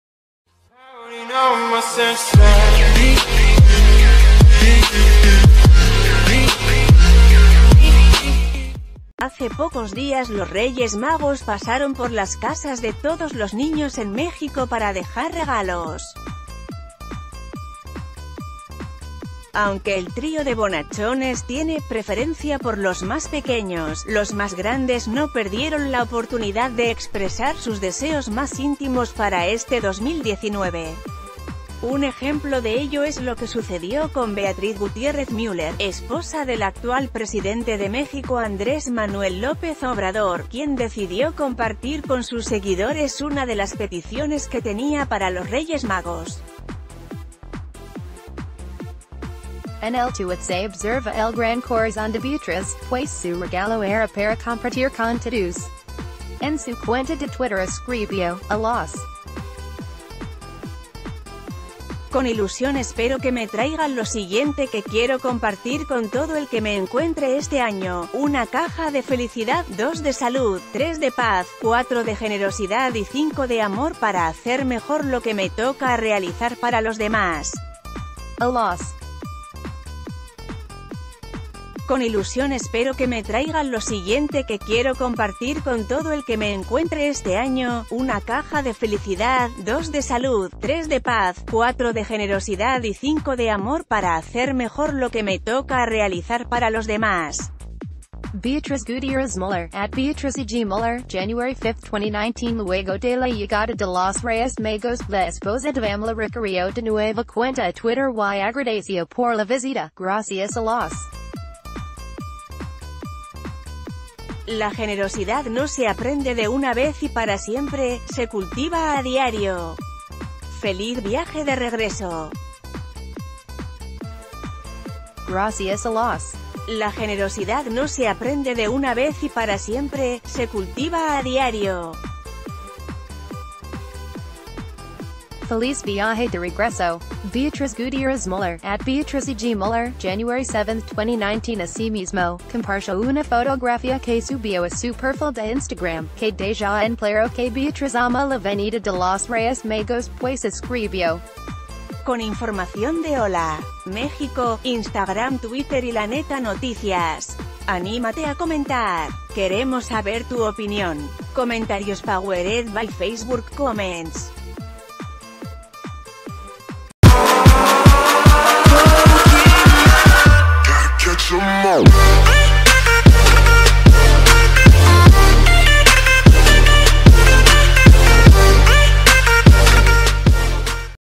Beatriz Gutiérrez Müller sorprende en redes con increíble carta a los Reyes Magos
Hace pocos días los Reyes Magos pasaron por las casas de todos los niños en México para dejar regalos. Aunque el trío de bonachones tiene preferencia por los más pequeños, los más grandes no perdieron la oportunidad de expresar sus deseos más íntimos para este 2019. Un ejemplo de ello es lo que suce...